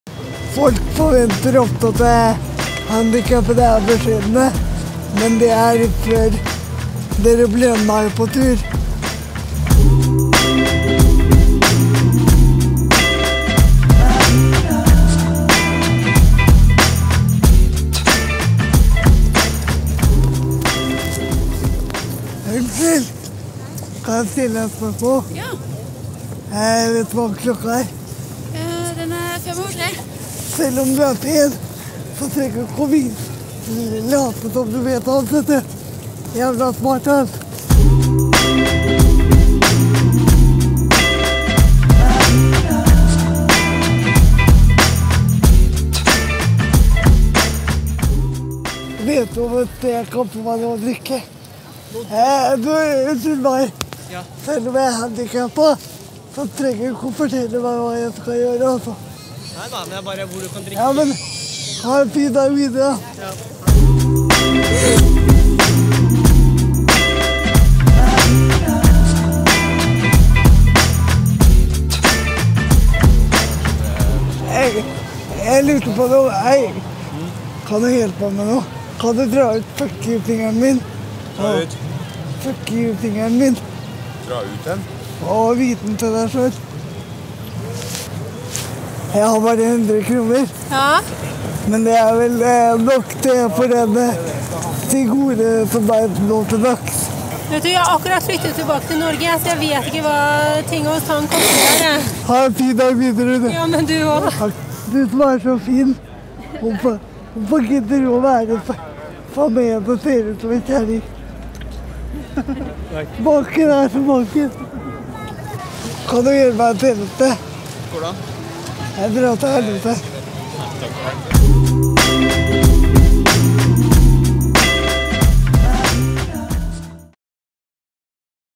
Rulletull med Ole Andreas.
Ole Andreas er lei av forskjellsbehandling og tar saken i egne hender.
Foto: Mads Kristiansen, Thomas Stenerud, Johannes Apelseth og Frans Graham Guttormsen.
Produsent: Lucky View.